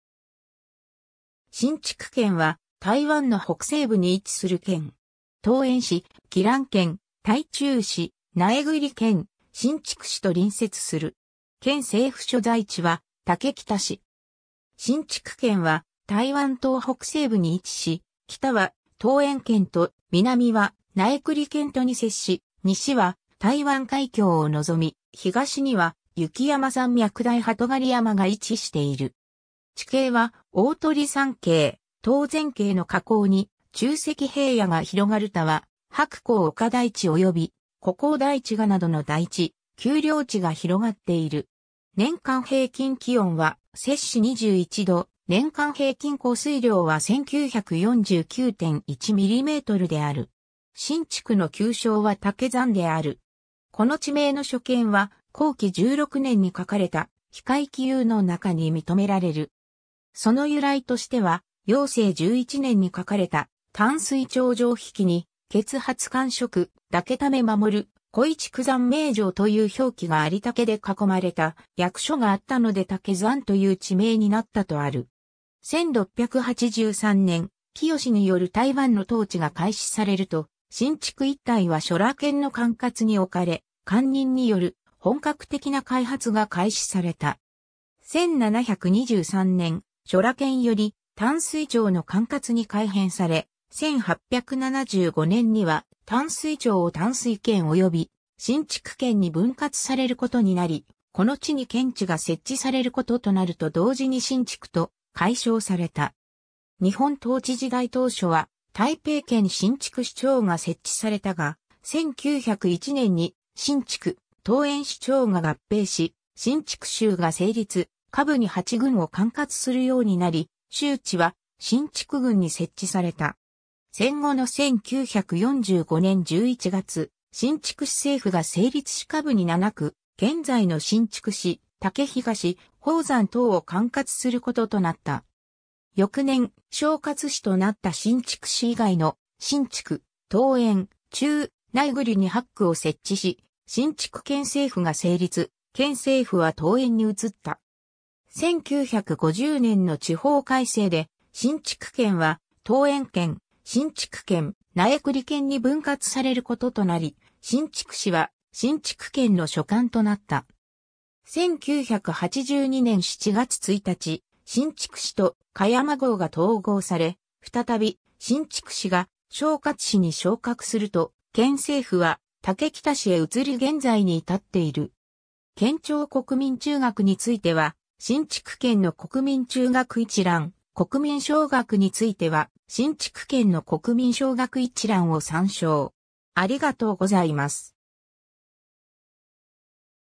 新竹県
新竹県（シンジュー/しんちく けん）は台湾の北西部に位置する県。
桃園市、宜蘭県、台中市、苗栗県、新竹市と隣接する。
県政府所在地は竹北市。
 新竹県は台湾島北西部に位置し、北は桃園県と、南は苗栗県とに接し、西は台湾海峡を臨み、東には雪山山脈大霸尖山が位置している。
地形は鳳山渓、頭前渓の河口に沖積平野が広がる他は伯公岡台地及び湖口台地がなどの台地、丘陵地が広がっている。
年間平均気温は摂氏21度、年間平均降水量は 1949.1 ミリメートルである。
 新竹の旧称は竹塹である。
この地名の初見は康熙16年（1677年）に書かれた『裨海紀遊』の中に認められる。
その由来としては雍正11年（1733年）に書かれた『淡水廳城碑記』に「厥初環植莿竹爲衞，故以竹塹名城」という表記があり竹で囲まれた役所があったので竹塹という地名になったとある。
 1683年、清による台湾の統治が開始されると新竹一帯は諸羅県の管轄に置かれ官人に依る本格的な開発が開始された。
1723年、諸羅県より淡水庁の管轄に改編され、1875年には淡水庁を淡水県及び新竹県に分割されることになり、この地に県治が設置されることとなると同時に新竹と改称された。
 日本統治時代当初は台北県新竹支庁が設置されたが、1901年に新竹、桃園支庁が合併し新竹州が成立、下部に8郡を管轄するようになり、州治は新竹郡に設置された。
戦後の1945年11月、新竹市政府が成立し下部に7区、現在の新竹市、竹東、宝山等を管轄することとなった。
翌年、省轄市となった新竹市以外の、新竹、桃園、中壢、苗栗に8区を設置し新竹県政府が成立、県政府は桃園に移った。
 1950年の地方改制で新竹県は桃園県、新竹県、苗栗県に分割されることとなり、新竹市は新竹県の所管となった。
1982年7月1日、新竹市と香山郷が統合され、再び新竹市が省轄市に昇格すると、県政府は竹北市へ移り現在に至っている。
 県長 国民中学については新竹県の国民中学一覧、国民小学については新竹県の国民小学一覧を参照